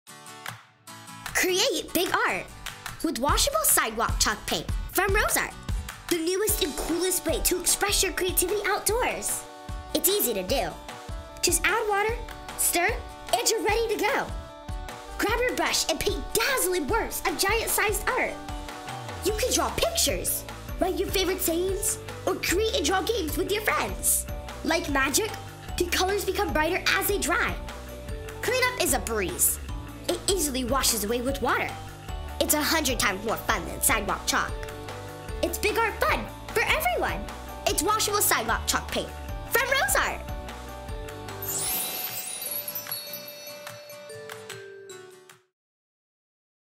RoseArt's Washable Sidewalk Chalk Paint In Action!!
Create BIG ART with Washable Sidewalk Chalk Paint by RoseArt! It's easy to do - just add water, stir and paint! It's 100 times more fun than sidewalk chalk. Clean up is a breeze too. It's big art fun for everyone.
An inexpensive outdoor art product to inspire play outside. All products under $20.  Great for easter baskets too.